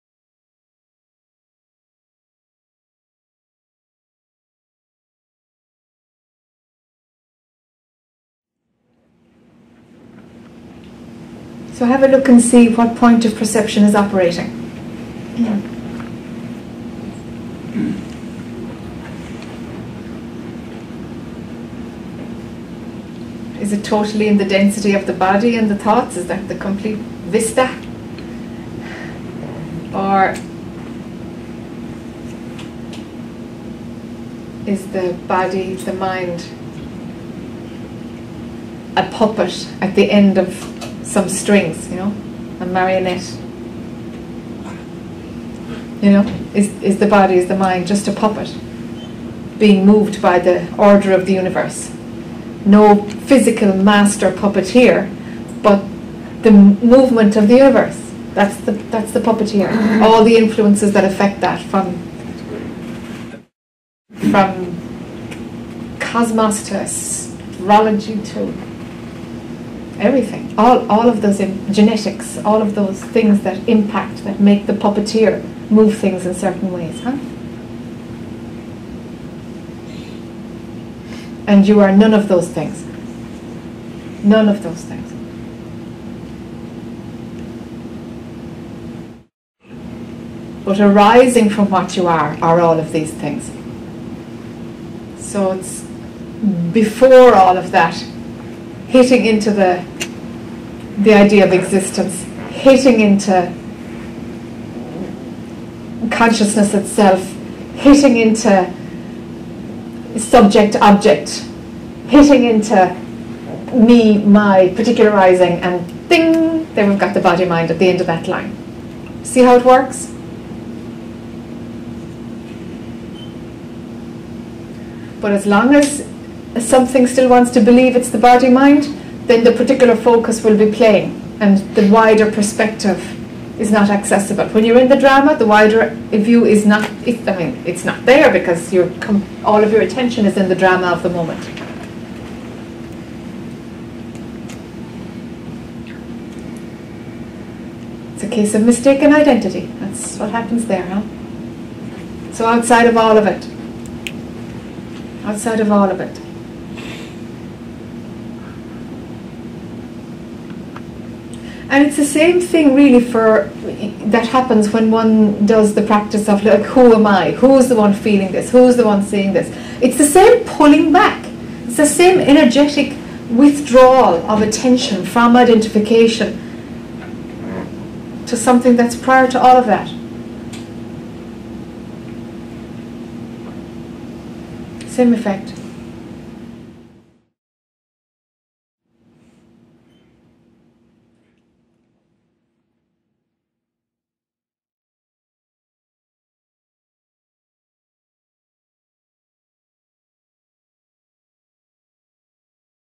Body mind is a puppet
Excerpt from Retreat with Jac O'Keeffe in Boston, October 2013